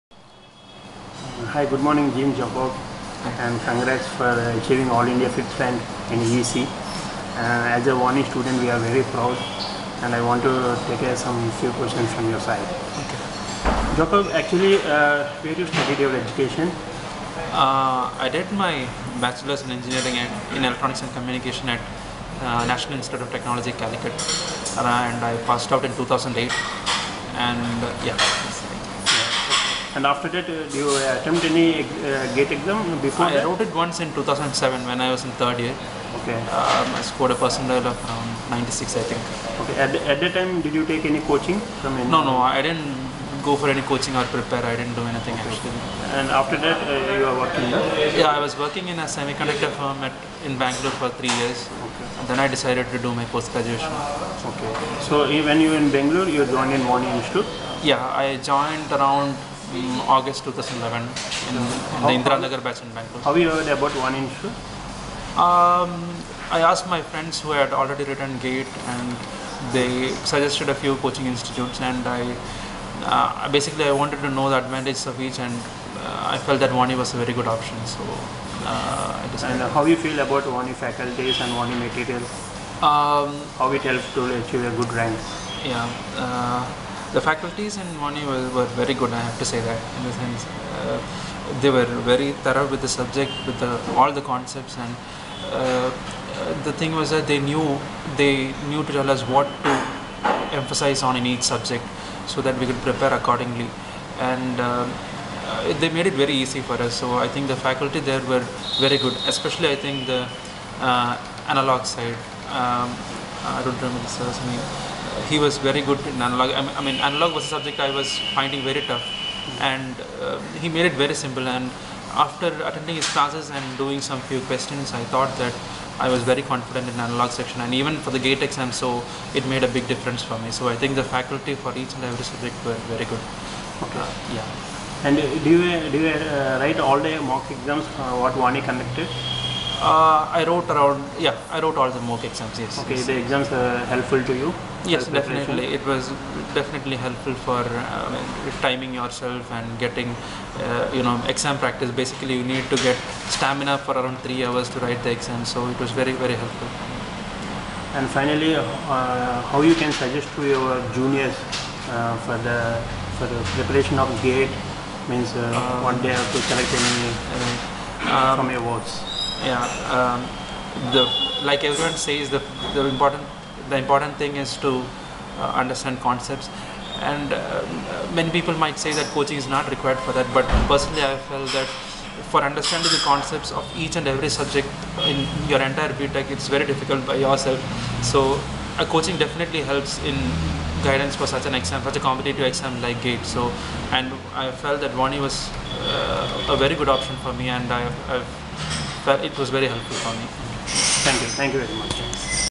INTERVIEW WITH VANI STUDENT WHO ACHIEVED ALL INDIA RANK 5 ECE IN GATE 2012
Vani Institute is well known as Gate & IES Coaching Centres In Hyderabad, Bangalore, Pune, Chennai, Vizag, Kochi, Kolkata, Nagpur and West Bengal.